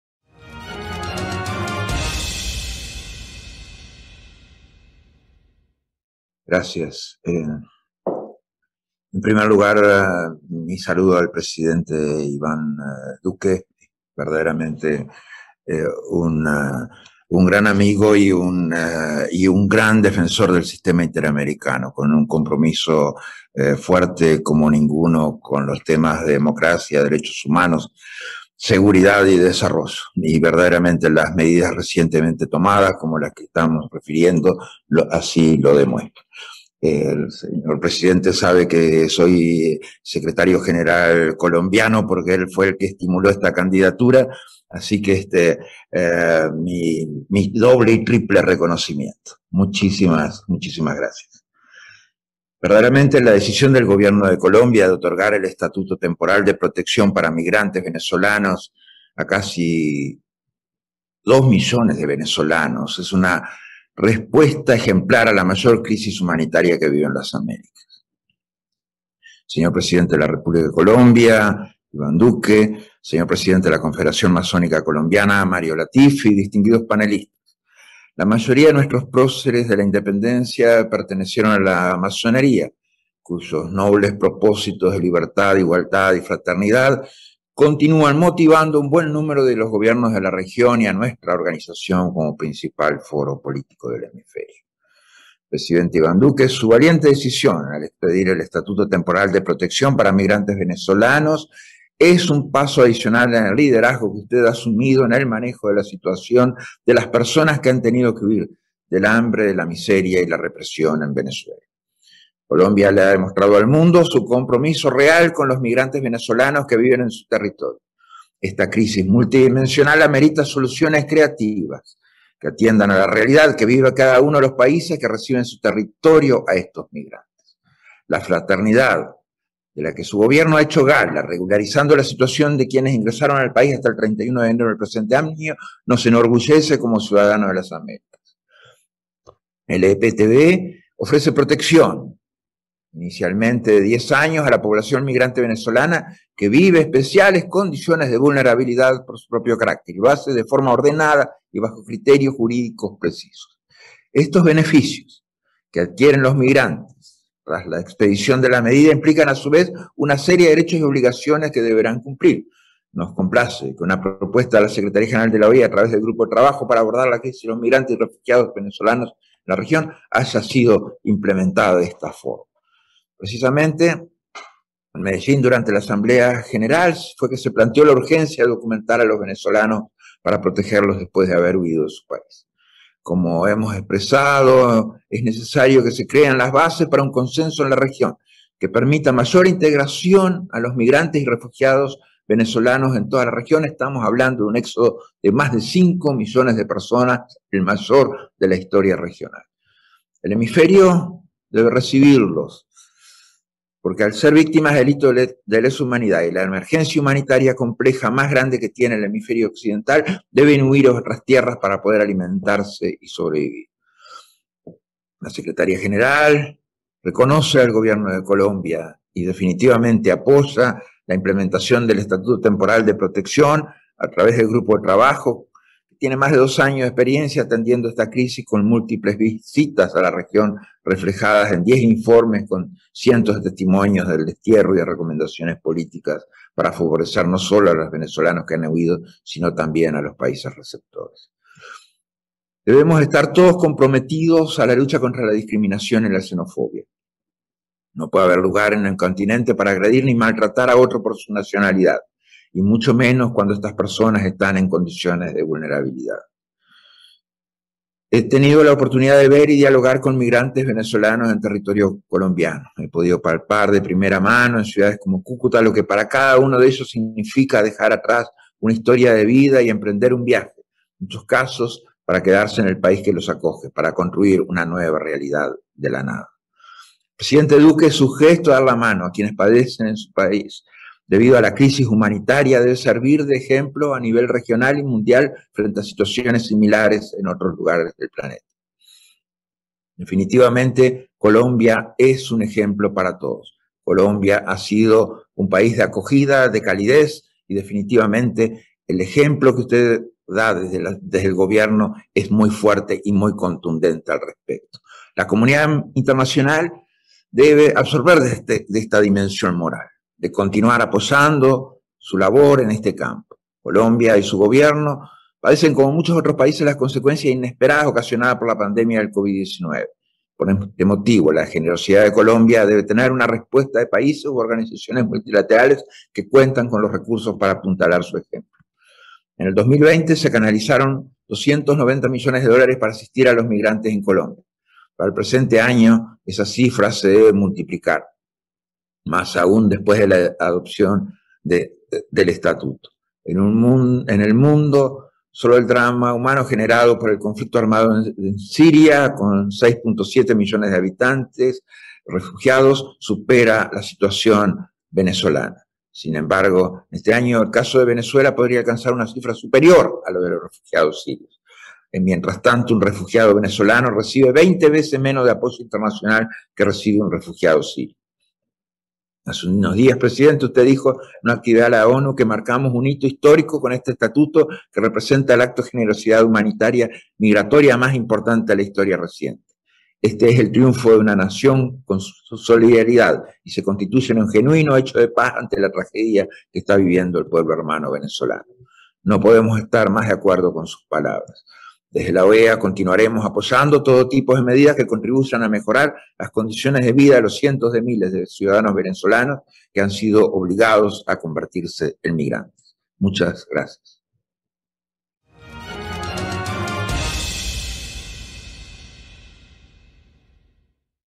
LLAMADO DE ALERTA AL MUNDO LUIS ALMAGRO 13MAR SOBRE ÉXODO VENEZOLANO
Llamamos a los países a seguir el ejemplo y crear consenso que permita mayor integración del éxodo venezolano, que con más de 5 millones es el mayor de la historia del continente.